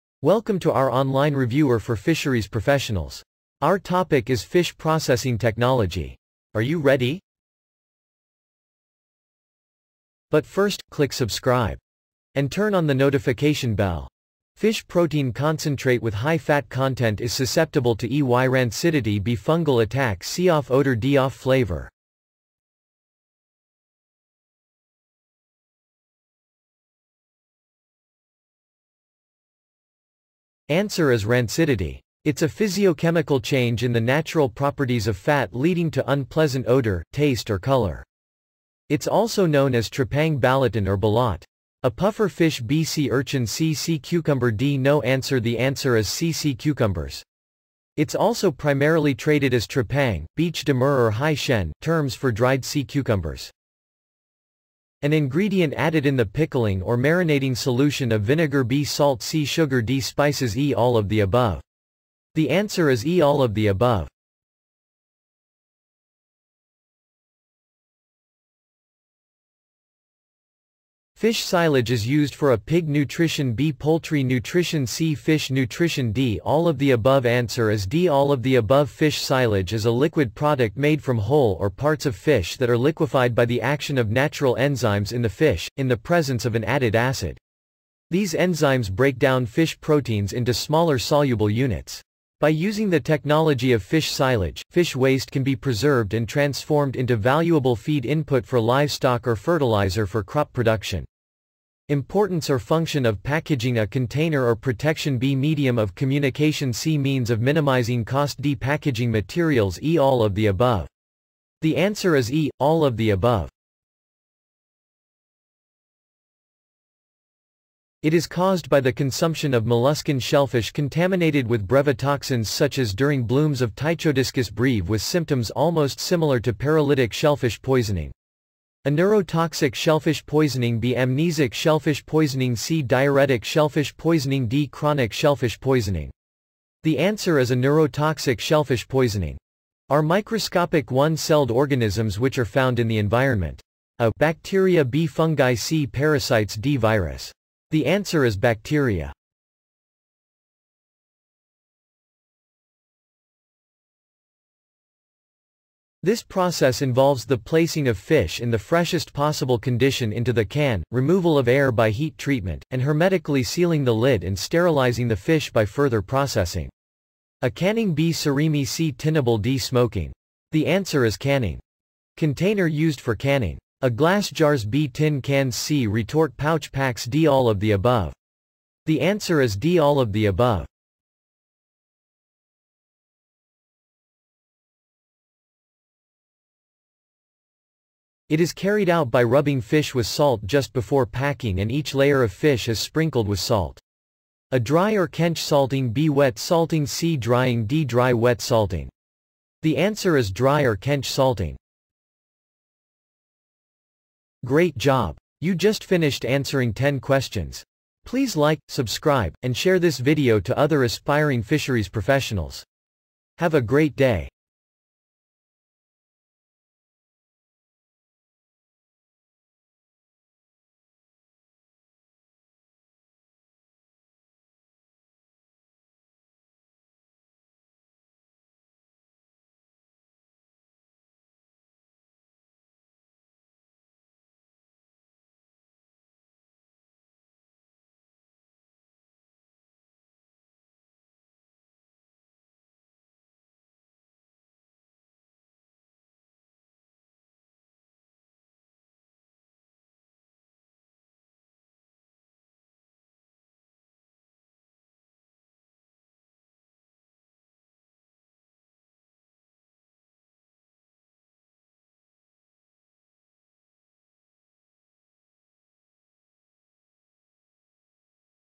Fisheries Board Exam Reviewer - Q54-63 TOPIC: Fish Processing Technology
This is a quick Q & A. Correction on #10 Question: It is carried out by rubbing fish with salt just before packing and each "LAYER" of fish is sprinkled with salt.